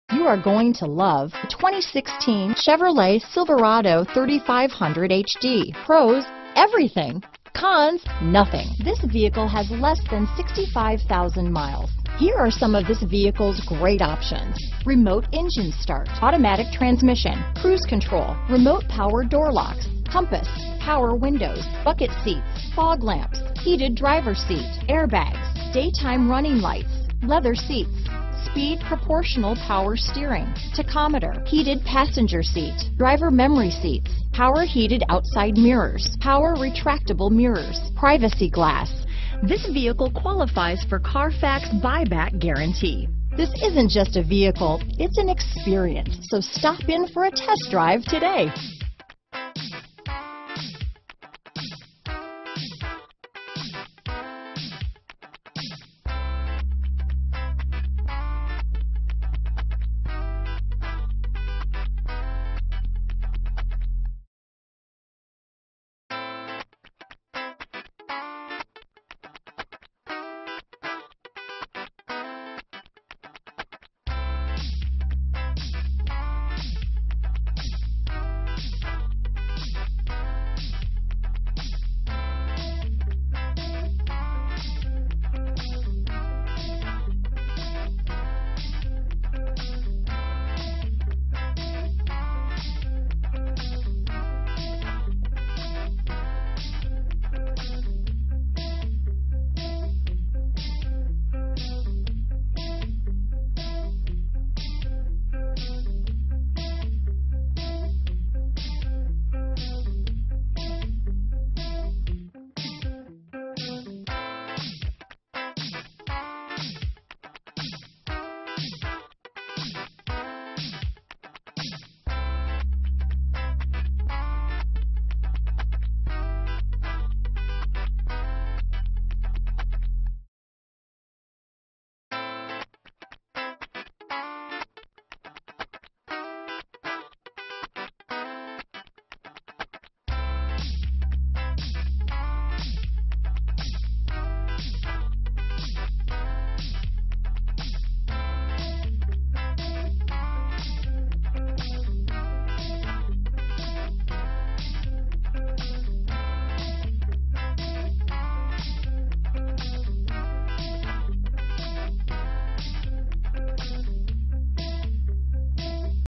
2016 Chevrolet Silverado 3500HD Lubbock, Amarillo, Plainview, Hereford, Clovis, TX PW1748A
Tungsten Metallic  2016 Chevrolet Silverado 3500HD available in Plainview, Texas at Plainview CDJR. Servicing the Lubbock, Amarillo, Hereford, Clovis, TX area.
2016 Chevrolet Silverado 3500HD LTZ - Stock#: PW1748A - VIN#: 1GC4K0CG6GF195050

Plainview Chrysler Dodge Jeep Ram
405 South I 27

Here it is! 4 Wheel Drive*** Includes a CARFAX buyback guarantee... Are you hunting for a car that you don't have to wonder if it will start in the morning? Well, this Truck will have you excited to pick-up even your mother-in-law.. Safety equipment includes: ABS, Passenger Airbag, Front fog/driving lights, Dusk sensing headlights, Daytime running lights...Other features include: Leather seats, Power locks, Power windows, Heated seats, Auto...
Wheels: 17' Forged Polished Aluminum, Front Full Feature Bucket Seats, Spare LT235/80R17E AT Tire, Integrated Trailer Brake Controller, Underbody Shield, LED Cargo Box Lighting, Smoked Amber Roof Marker Lamps, Engine: Vortec 6.0L VVT V8 SFI, Front License Plate Kit, Preferred Equipment Group 1LZ, (0 P) Tungsten Metallic, 17' x 6.5' Full-Size Steel Spare Wheel, 4 Doors, 4-wheel ABS brakes, 4WD Type - Part-time, 8-way power adjustable drivers seat, 8-way power adjustable passenger seat, AC power outlet - 1, Air conditioning with dual zone climate control, Audio controls on steering wheel, Automatic Transmission, Climate controlled - Driver and passenger heated-cushiondriver and passenger heated-seatback, Compass, Cruise control, Daytime running lights, Driver memory seats, Dusk sensing headlights, External temperature display, Four-wheel drive, Front fog/driving lights, Heated drivers seat, Heated passenger seat, Intermittent window wipers, Leather seats, Limited slip differential - Mechanical, Memory settings for 2 drivers, Multi-function remote - Trunk/hatch/door/tailgateremote engine start, Overhead console - Mini with storage, Passenger Airbag, Power heated mirrors, Power retractable mirrors, Power windows with 4 one-touch, Privacy/tinted glass, Remote engine start, Remote power door locks, Signal mirrors - Turn signal in mirrors, Speed-proportional power steering, Split-bench rear seats, Tachometer, Tilt and telescopic steering wheel, Universal remote transmitter